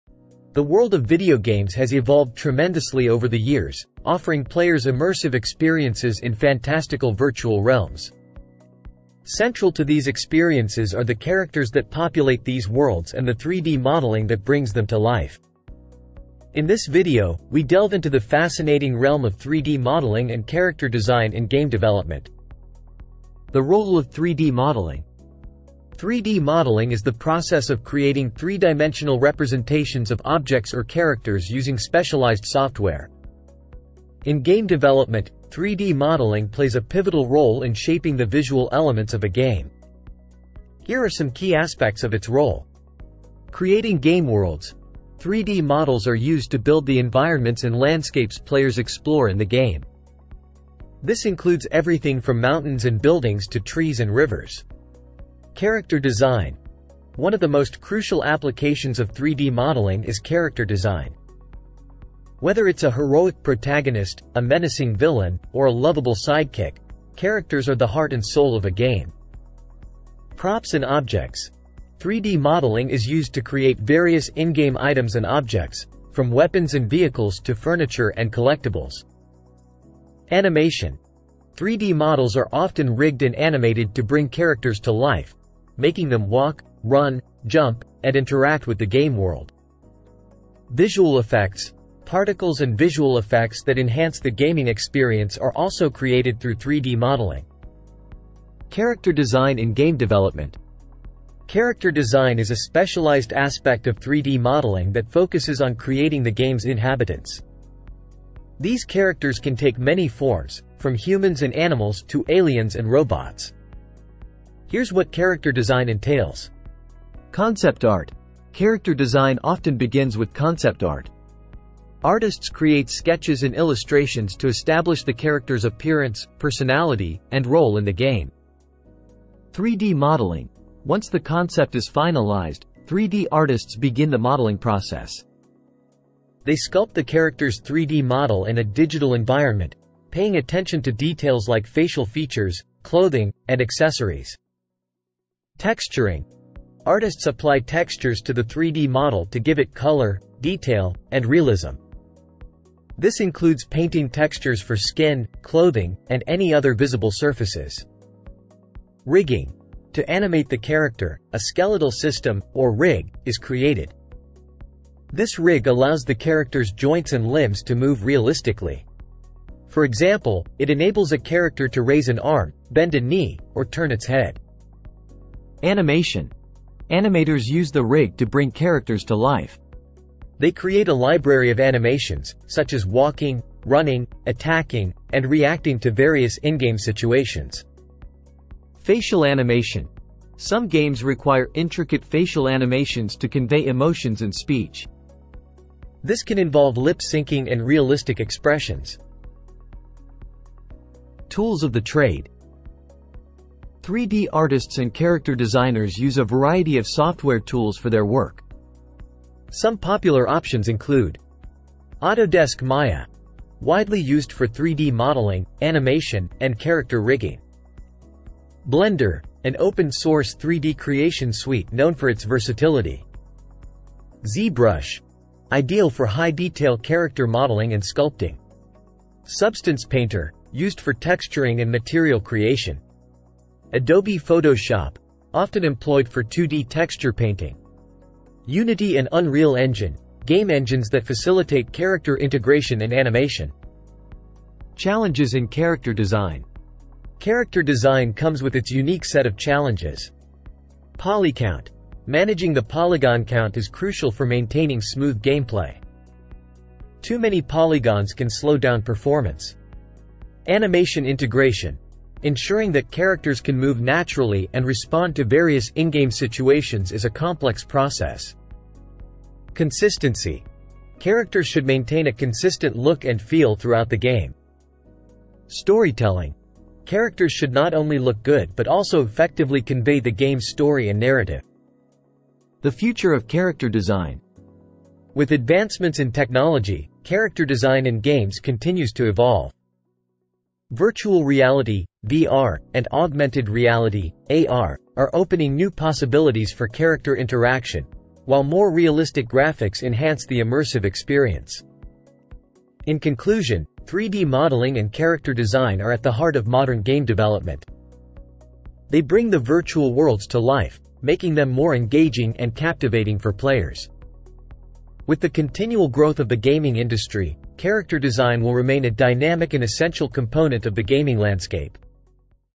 3D Modeling and Character Design in Game Development
Delve into the art of 3D modeling and character design in the realm of game development. Learn how designers bring game characters to life through intricate 3D modeling.